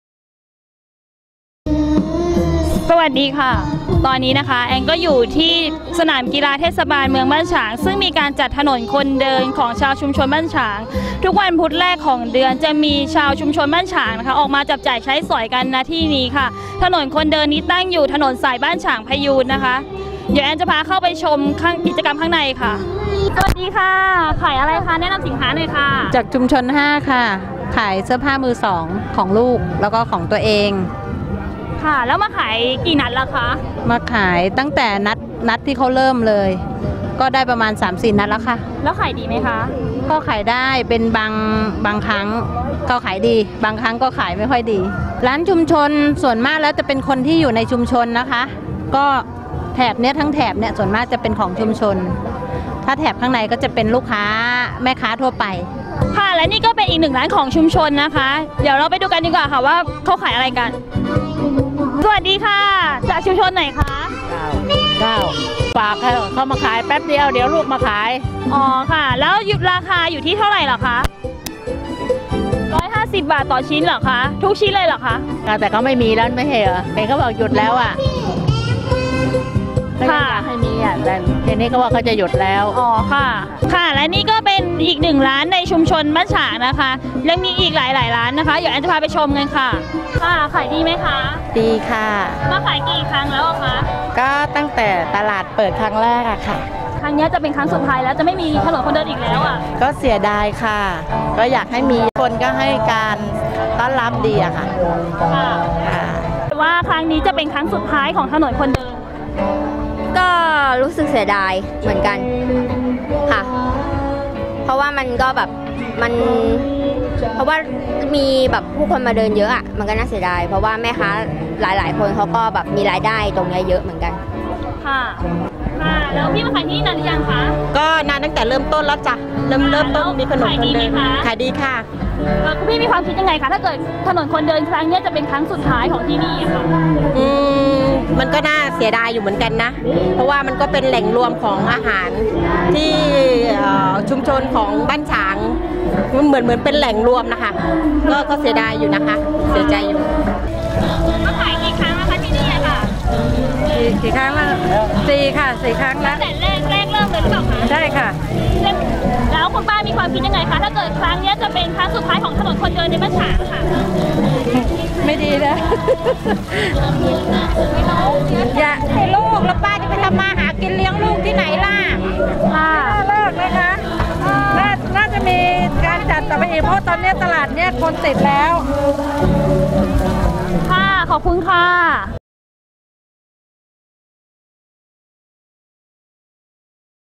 ตลาดนัดถนนคนเดินเมืองบ้านฉาง
แม่ค้าชุมชนรู้สึกเสียดาย ปิดดำเนินกิจกรรมตลาดนัดถนนคนเดิน เมืองบ้านฉาง ซึ่งเมื่อวันที่(๖/๕/๕๘)จัดขึ้นเป็นนัดสุดท้าย โดยเทศบาลเมืองบ้านฉางมีแนวคิดปรับปรุงสวนปรับปรุงสวนสุขภาพที่มีพื้นที่ดูแล ๒๔ ตารางเมตร โดยปกติวันใหนไม่มีกิจกรรมถนนคนเดินพบว่า มีประชาชนมาใช้ประโยชน์กันน้อยจึงได้คิดปรับปรุงใหญ่ใช้งบ ๒๐ กว่าล้านบาท เพื่อเป็นสถานที่พักผ่อนออกกำลังกายและส่งเสริมท่องเที่ยวขายสินค้าด้านหน้าของสวนสุขภาพ เพื่อเป็นรายได้เข้าชุมชนและส่งเสริมชุมชน จะปรับปรุงเพื่อให้ทุกคนมาใช้ประโยชน์เพิ่มขึ้น คาดจะแล้วเสร็จปลายปีนี้และเสร็จสมบูรณ์ทั้งหมดในต้นปี๒๕๕๙